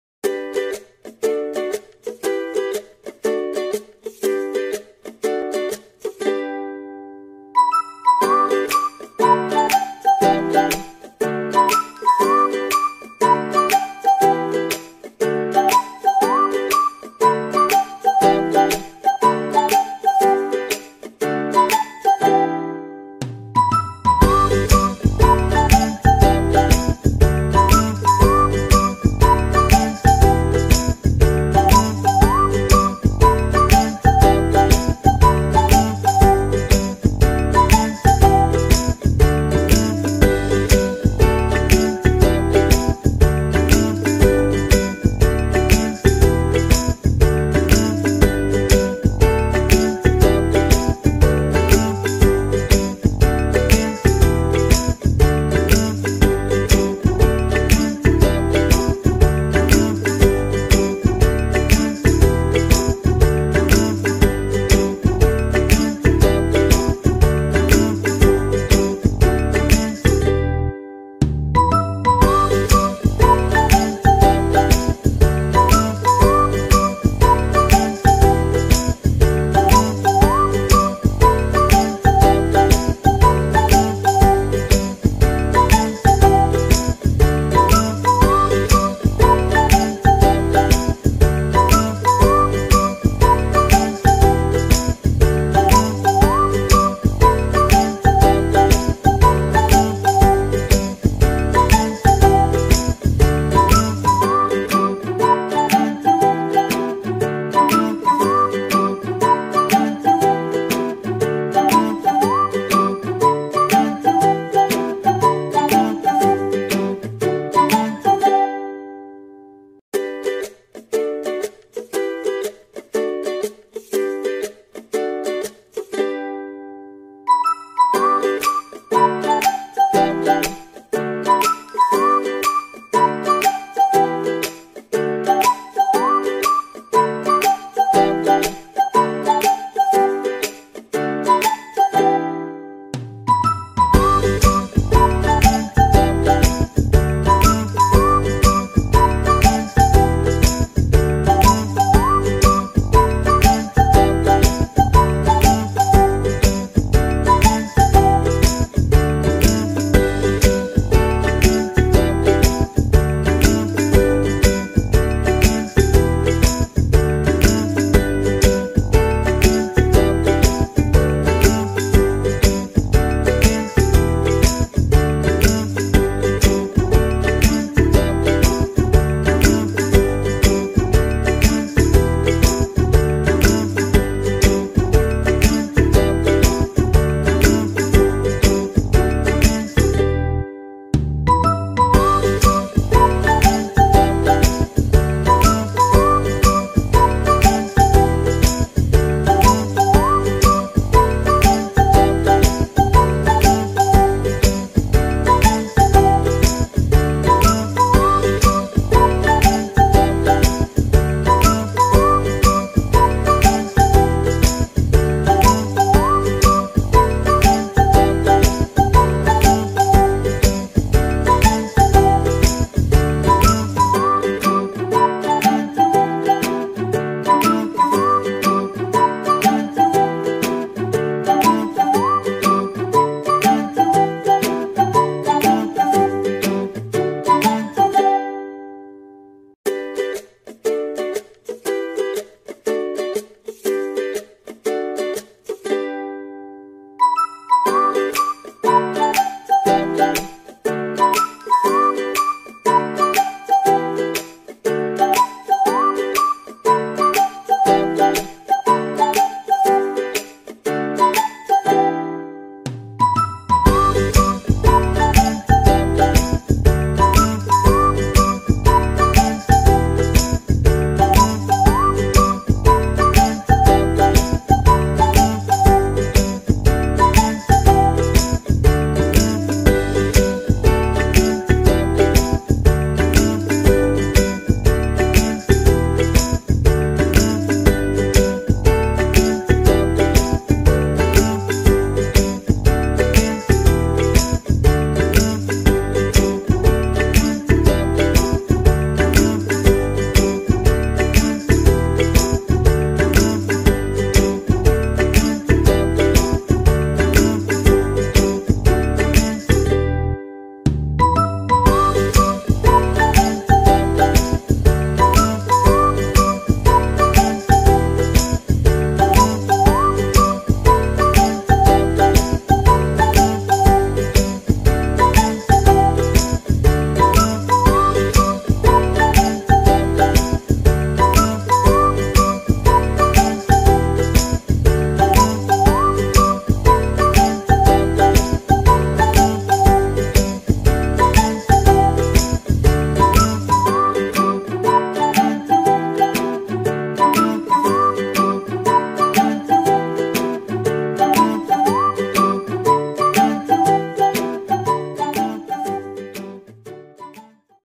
Siomai Sardines (MADE FROM TYPHOON ULYSSES FOOD PACK)
Siomai or Shumai is a traditional chinese dumpling. In Cantonese cuisine, it is usually serve as dim sum snack. Traditional made from pork but later on developed various flavours. Try this new flavour and let me know what you think down on the comment box!